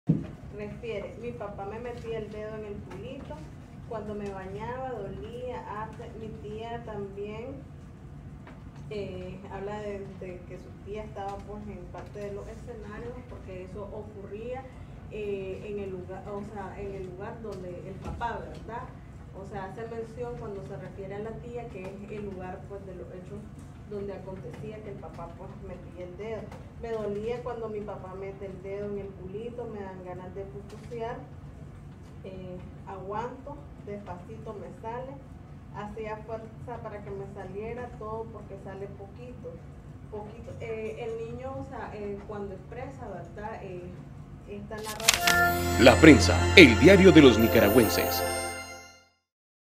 Psicóloga se pronuncia en el caso de violación
Según la Psicóloga María Raquel Aráuz los niños reflejan vocabulario sexual no adecuado para su edad, más trastornos de ansiedad y comportamiento alterado.